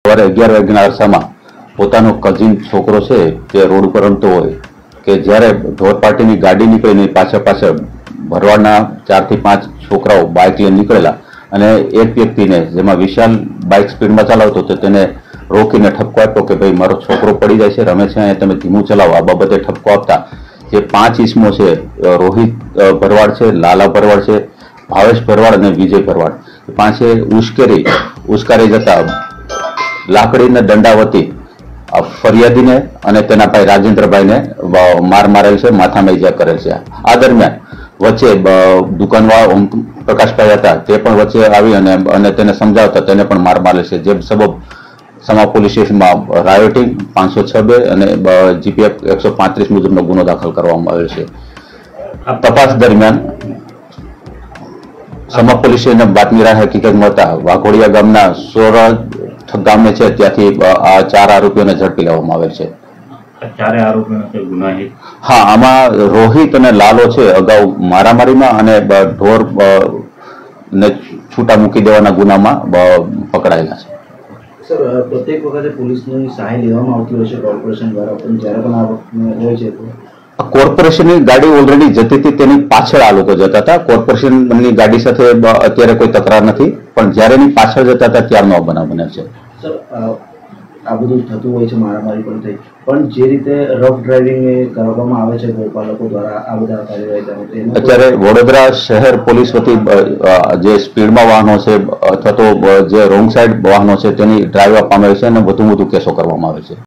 Breaking News: Sama Police Station Addresses Rioting in Sama Area
"Catch the exclusive press conference by Sama Police Station addressing the recent riots in Sama area of Vadodara. Stay informed on the latest updates and insights from the authorities. SamaPolice PressConference VadodaraNews"